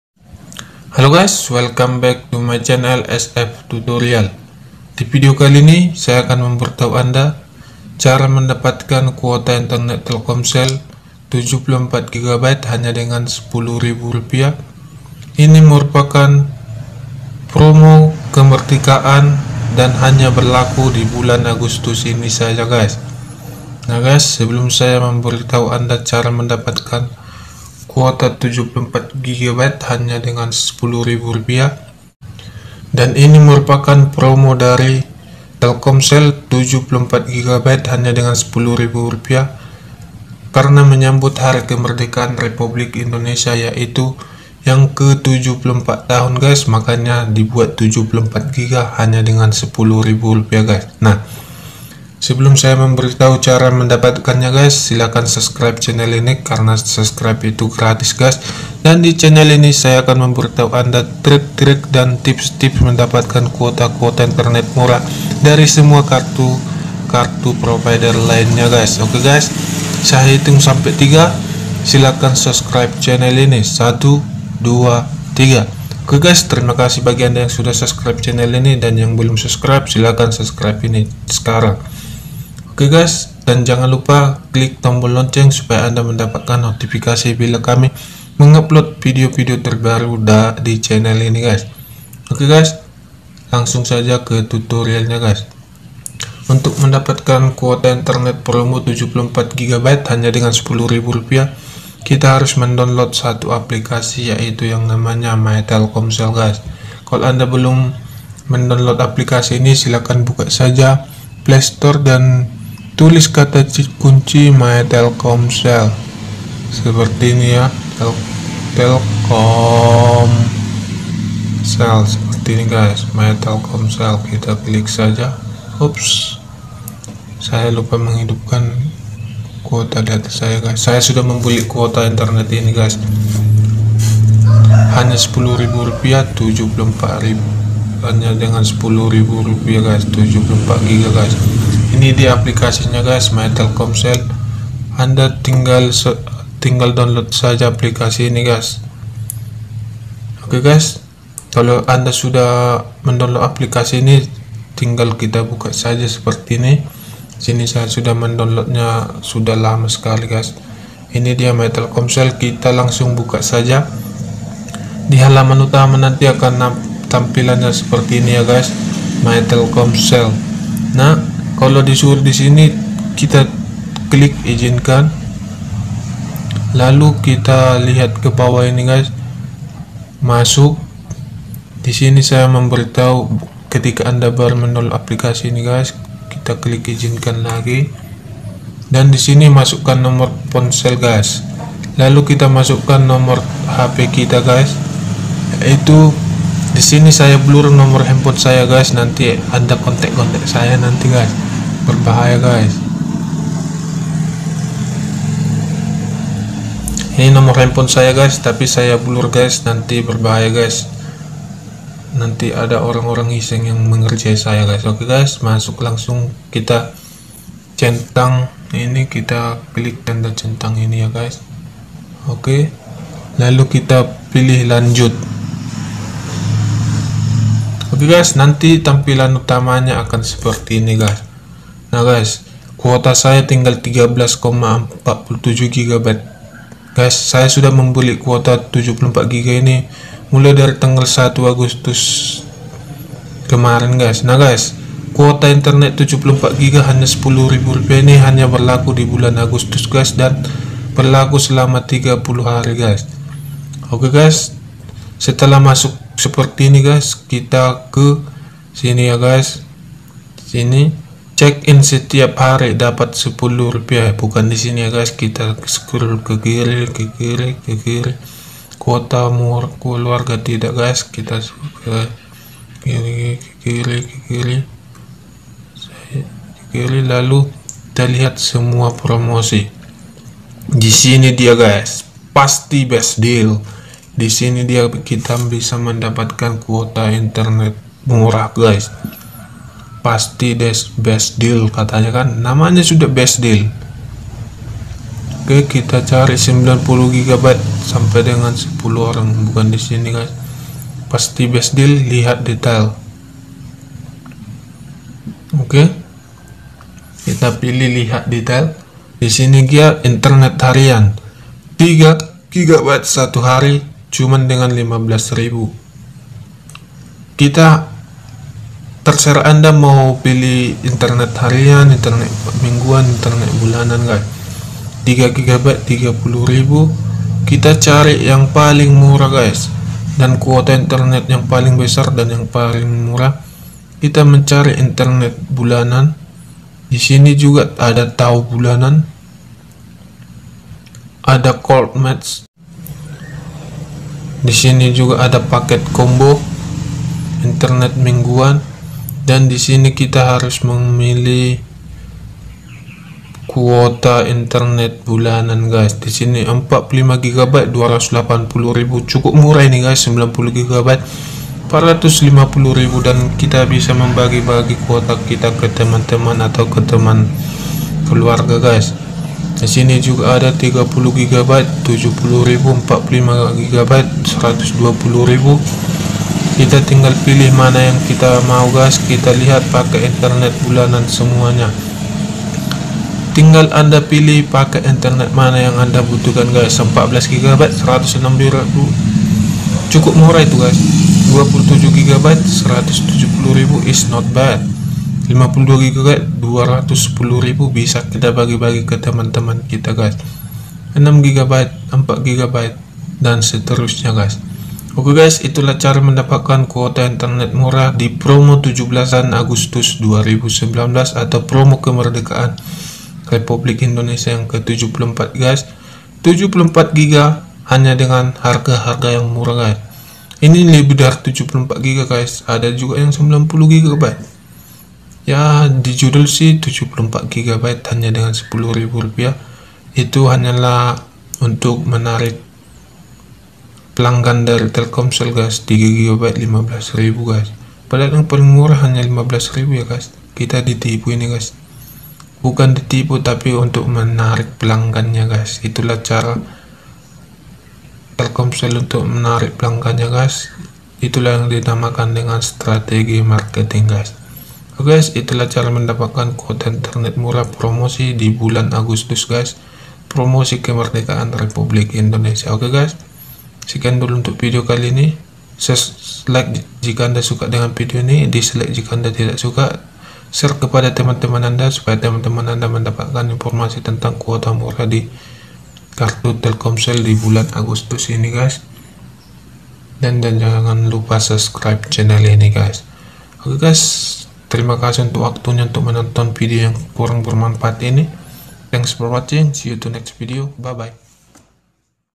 WOW! 74GB Cuman 10rb Promo Kemerdekaan RI Ke74
Kode Deal Kuota Internet Murah Telkomsel Promo Keemerdekaan RI Ke74 hanya berlaku di bulan Agustus 2019.